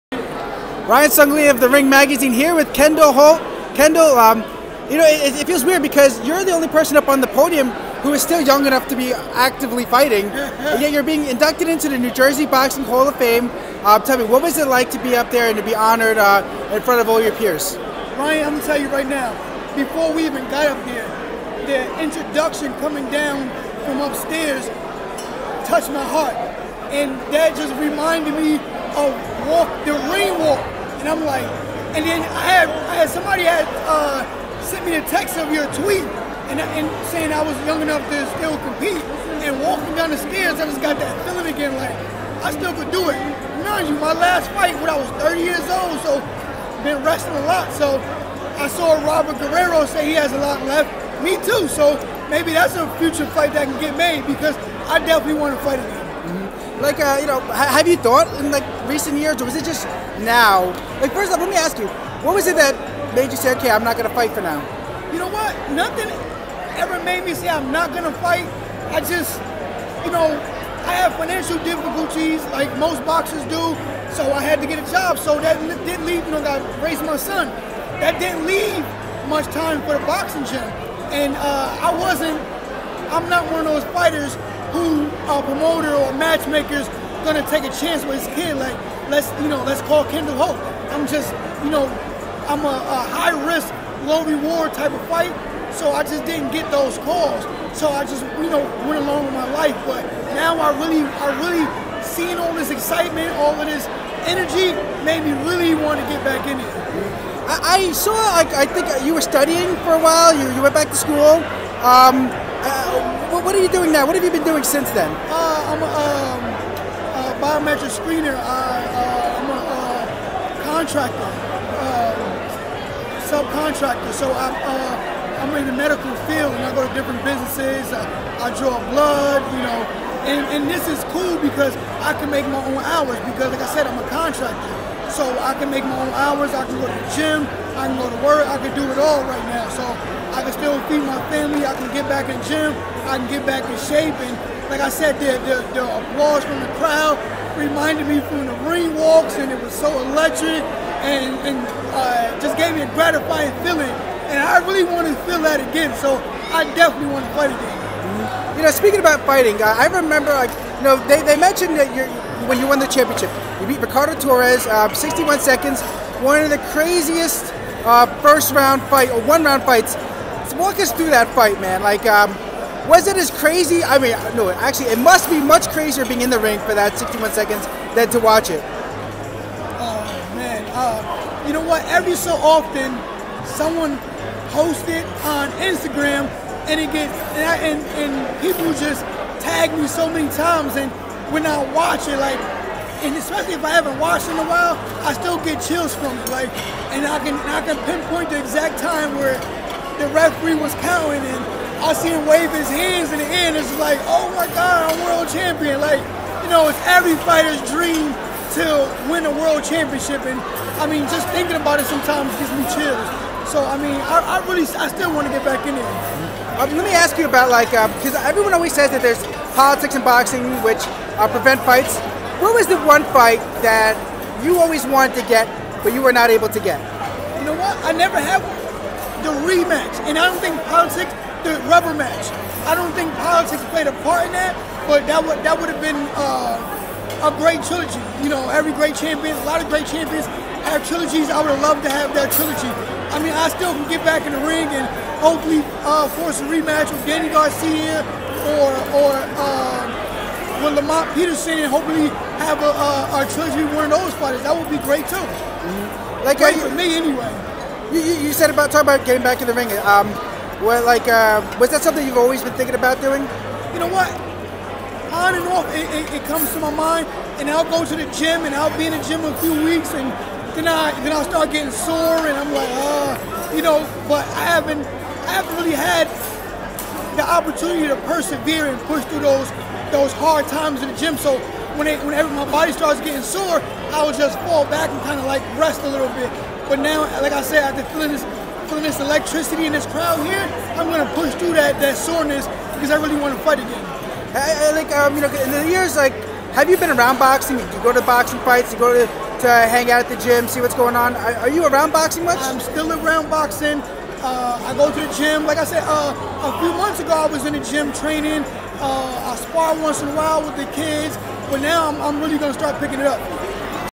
Kendall Holt considering ring return after 5 years
Former WBO junior welterweight champ Kendall Holt speaks to The Ring about his induction into the NJ Boxing Hall of Fame, going back to college and returning to the ring. Interview by Ryan Songalia.